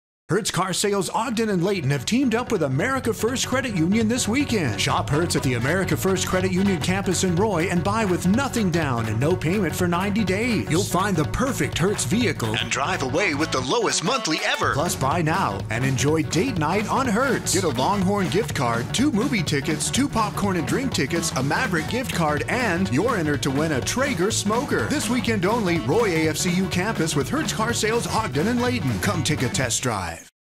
Hertz Car Sales Credit Union Sale
IT’S ON! Hertz Car Sales Ogden and Layton have teamed with America First Credit Union to save you thousands through Saturday Night! Shop Hertz at the AFCU Campus in Roy (4051 S. 1900 W. Roy, UT) and buy with nothing down, make no payments for 90 days and get rates as low as 2.99% apr/oac. Buy this weekend, and enjoy date night on Hertz. Get a Longhorn Gift Card, 2 movie tickets, 2 popcorn and drink tickets, a Maverick Gift Card and... You're entered to win a Traeger Smoker! Through Saturday (9/21) only—with AFCU and Hertz Car Sales.